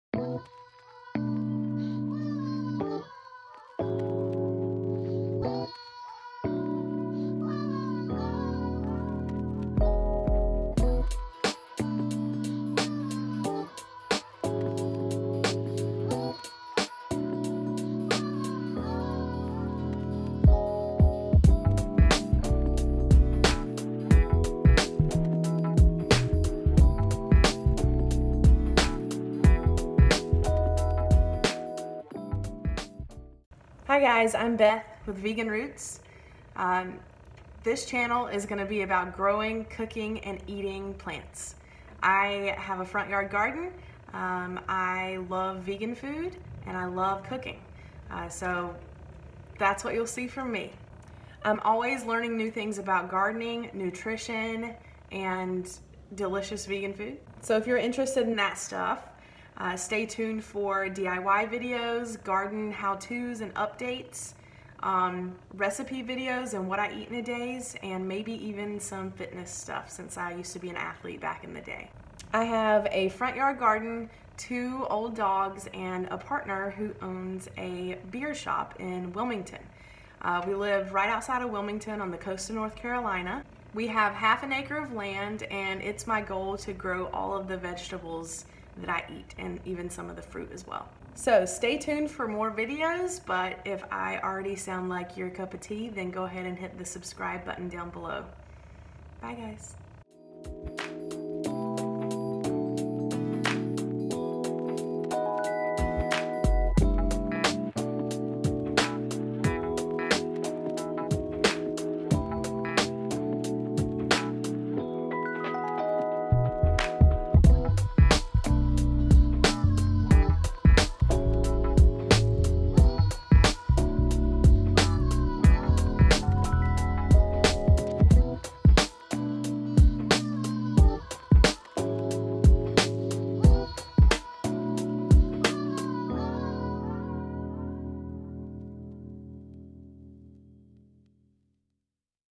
Welcome to My Channel!
Hello, fellow plant lovers! I'm an amateur gardener in coastal North Carolina. I'm passionate about vegan food, sustainability, and growing what you eat. You can expect gardening updates and how-tos, recipe and what-I-eat videos, and the occasional vlog (and subsequently doggie videos). Happy growing!